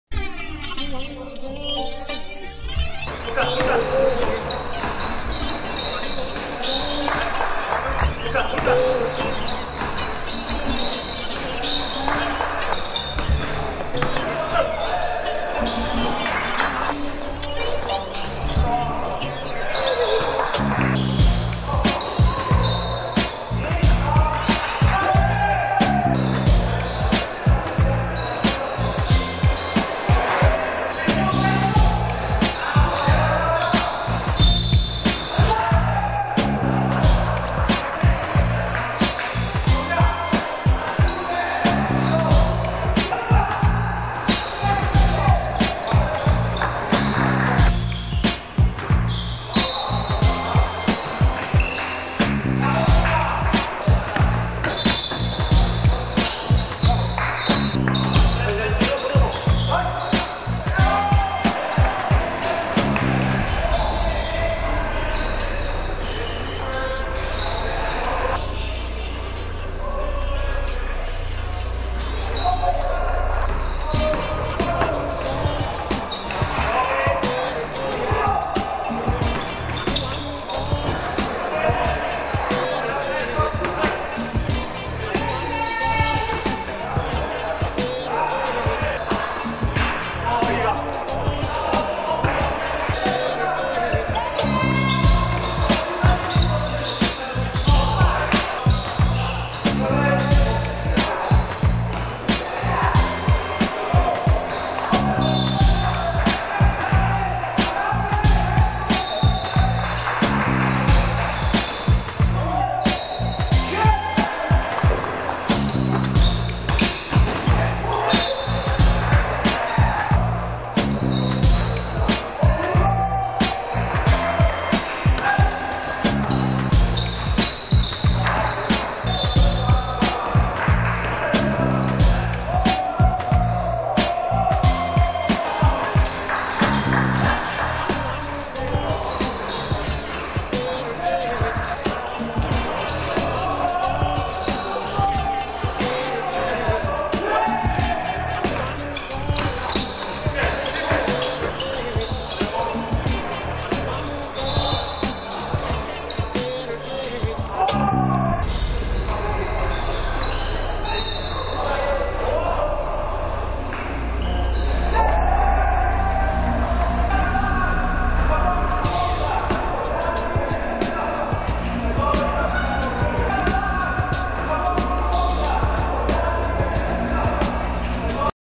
05 06 Seattle vs Utah – Draft Liga 9
SEATTLE 101:64 UTAH
M.Wołyniec 32pkt 4x3 5zb 8ast 4stl
K.Michałek 27pkt 3stl